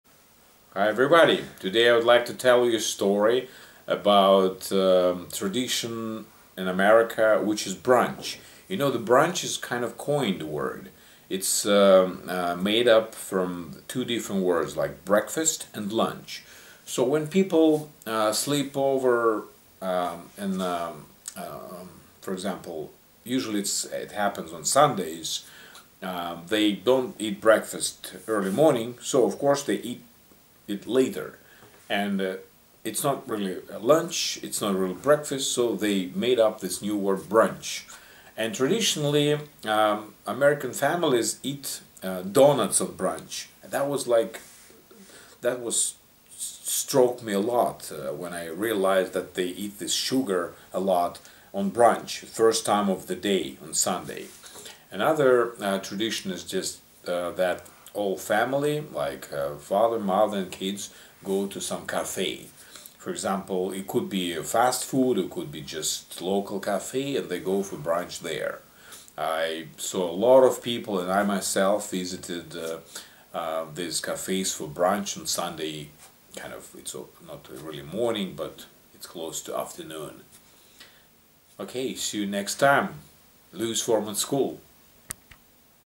40. Bruch
Today Nick is talking about a brunch tradition.